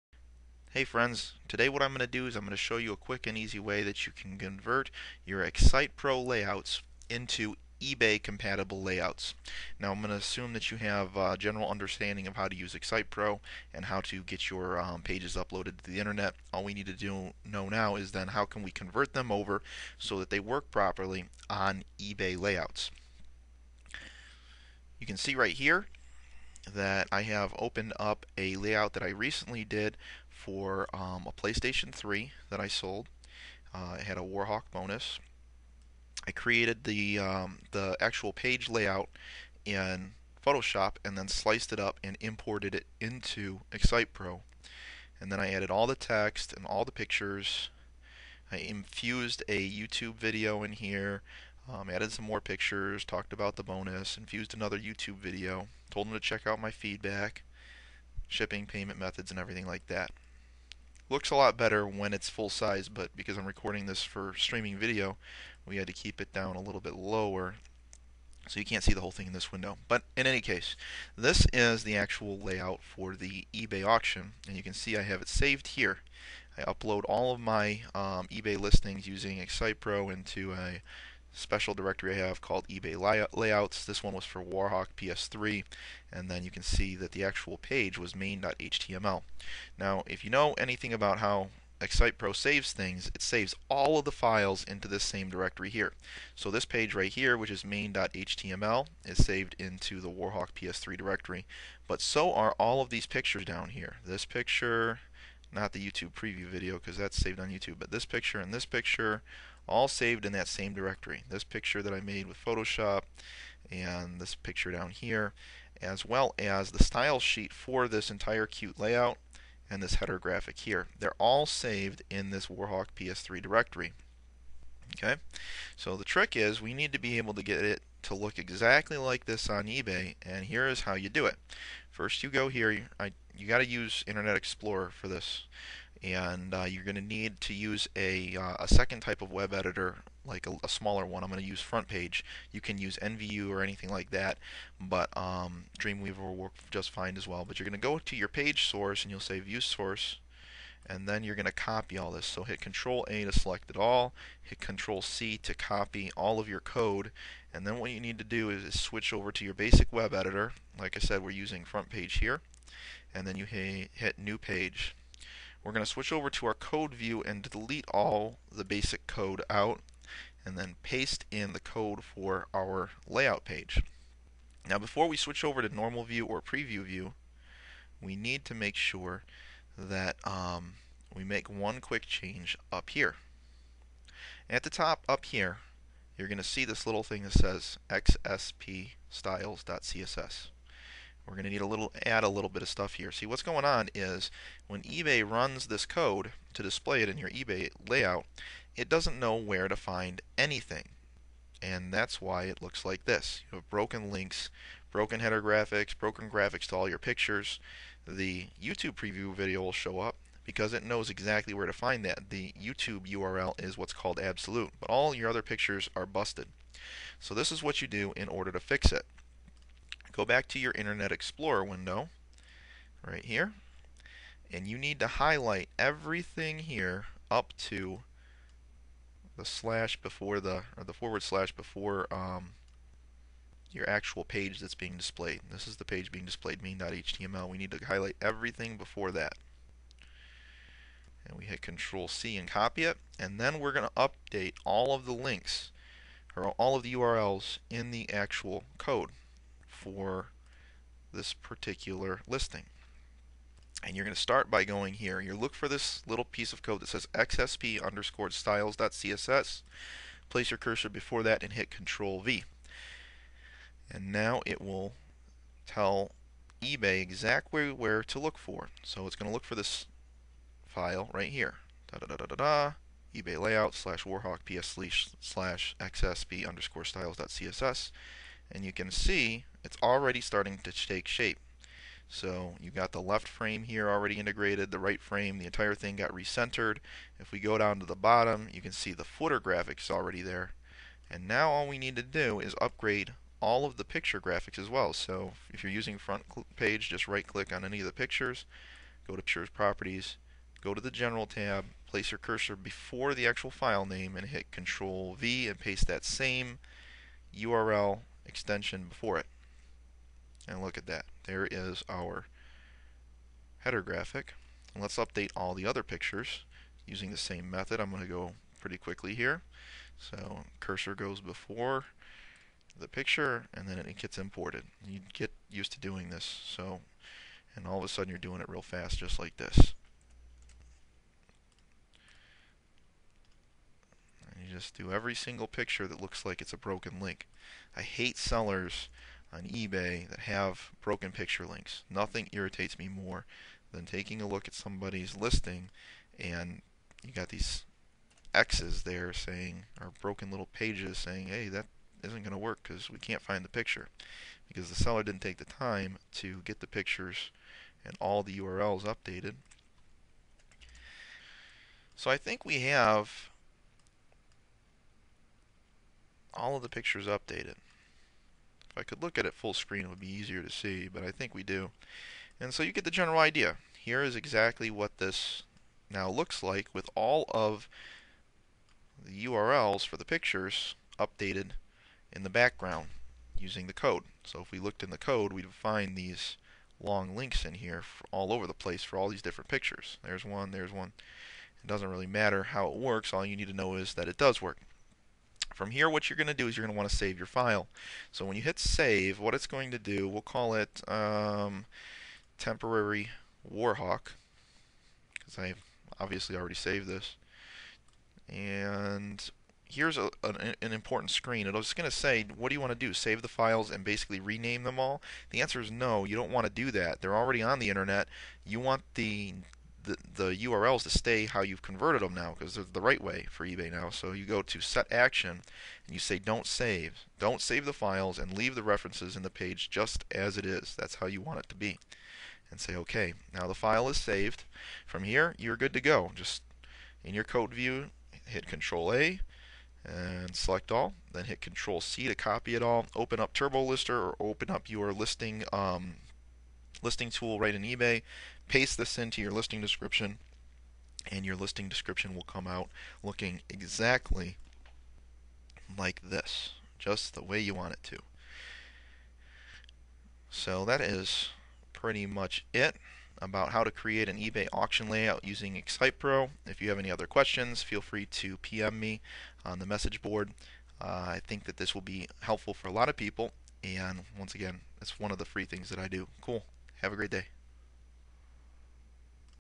Convert XSitePro Layouts to Ebay Auction Listing Templates
How to convert your XSite Pro Layouts to Ebay Auction Listings and still have the formating and pictures show up properly.